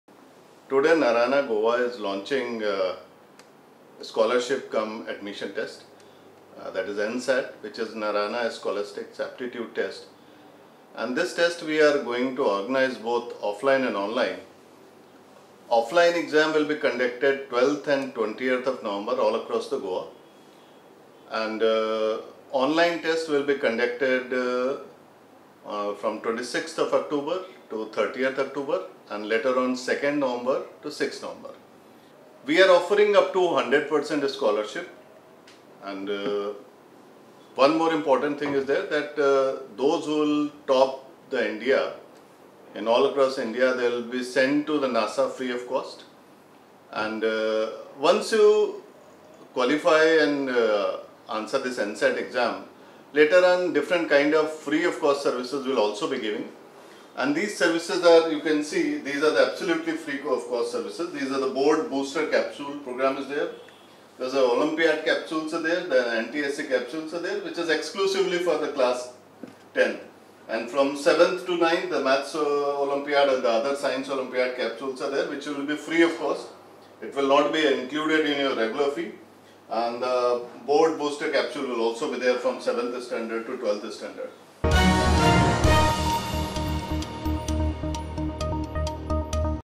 NARAYANA GROUP ANNOUNCES TALENT HUNT EXAM NSAT, TOPPER WINS FREE TRIP TO NASA
The Narayana Institute of Coaching has announced the Narayana Scholastic Aptitude Test, known as N-SAT. The test is one of the biggest talent hunt exams conducted in Goa. The test will be conducted in both online and offline mode. The online test will be conducted from 26 to 30 October and 2 to 6 November. The offline test will be held on 12 and 20 November. The topper of the test stands a chance to win a free trip to NASA. Anuj Kumar Singh, Goa head informed that students will get 100 per cent scholarships along with other free courses. The Narayana group also announced various capsules for students. The group trains students for IIT-JEE, NEET and other competitive exams.